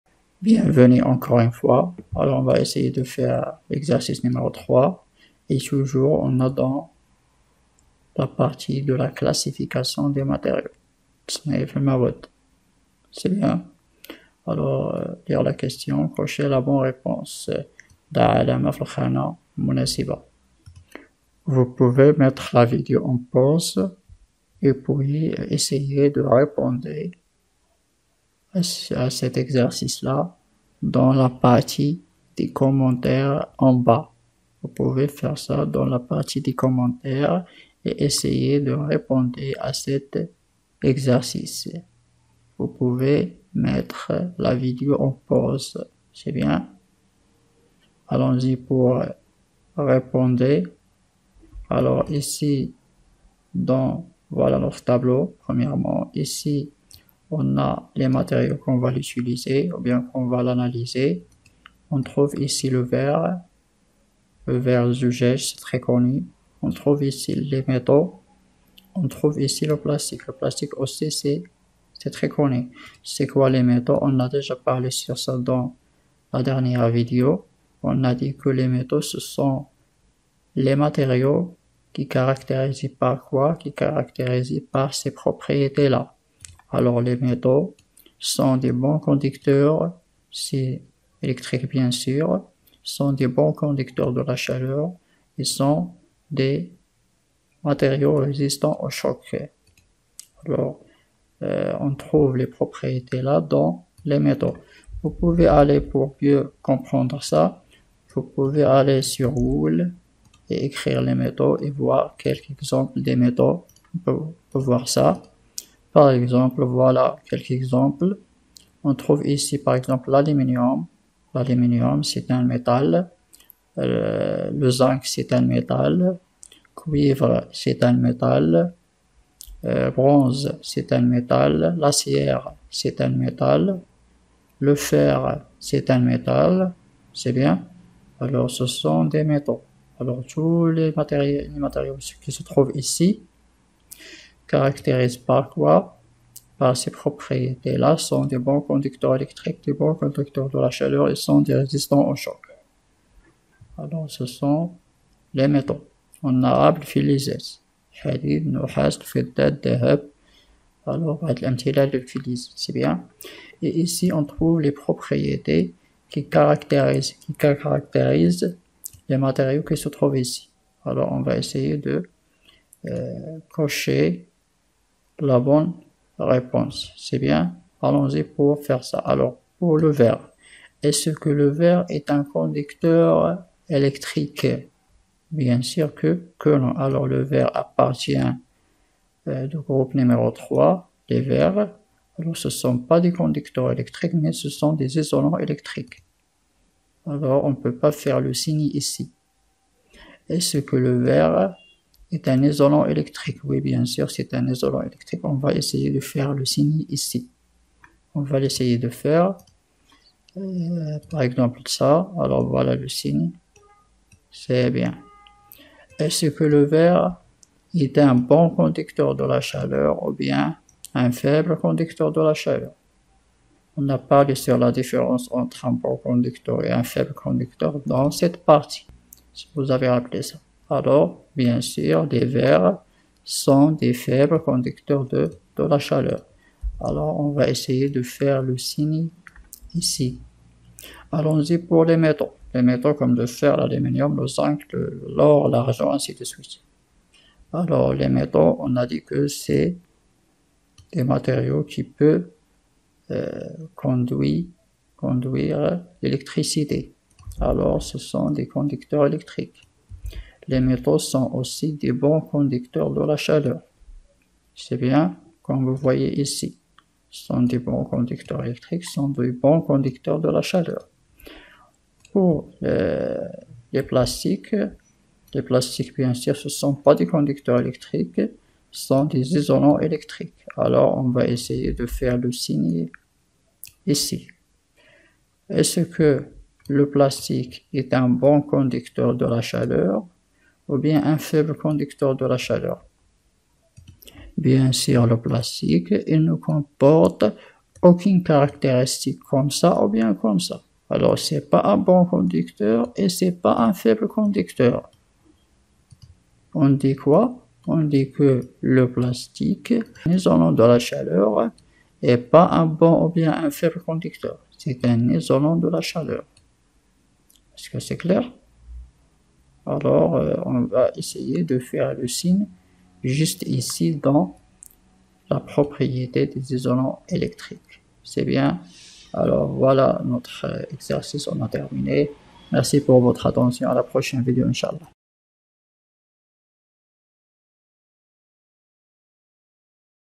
11 - Exercice 3 - La classification - Exemples de quelques matériaux utilisés dans la vie - 3APIC
La classification des Materiaux - Exercice objet et materiaux - Exemples de quelques matériaux utilisés dans la vie quotidienne - مسلك دولي الثالثة اعدادي - quelques matériaux au quotidien
By : Prof. AMARI ALAE - COLLEGE ALJOULANE - RABAT
Contactez Moi : Easycours.com/contact
Cours : .
Exercices : .